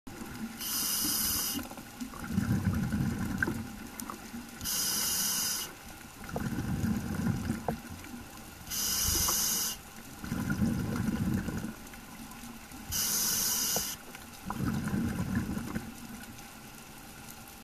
Biofluorescent Night Dive - Anemone with clown fish (Dahab/Egypt)
for more information about underwater fluorescence and necessary equipment.

Equipment:
Hartenberger maxi compact LCD torch with 21 x blue LED head and NightSea BE1 dichroic excitation filter
Nikon CoolPix P300 camera with Ikelite UW housing and NightSea BB67 yellow barrier filter

For more information about fluorescence-diving and necessary equipment see also